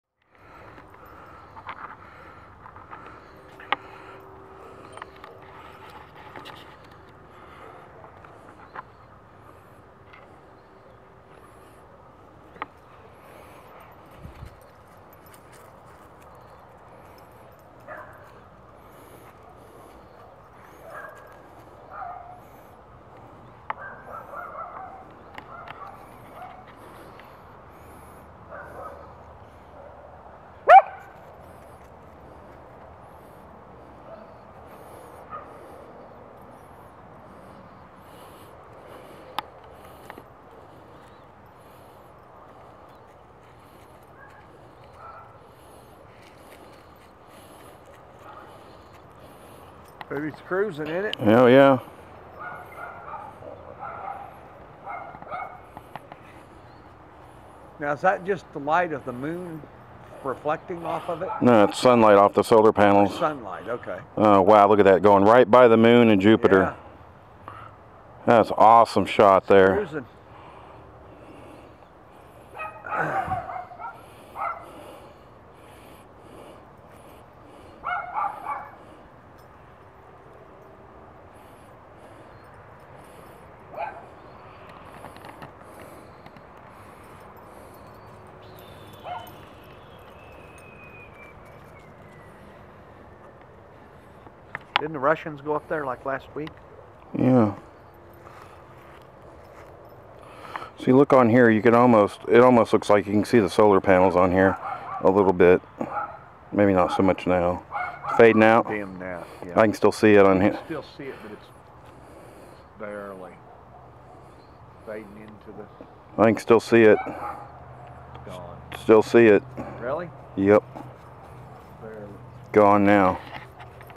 ISS 01 02 12 International Space Station Pass
ISS Pass from 2012 over Northeast Kansas.  Shot with Nikon D5100 and Tamron 70-200 F2.8.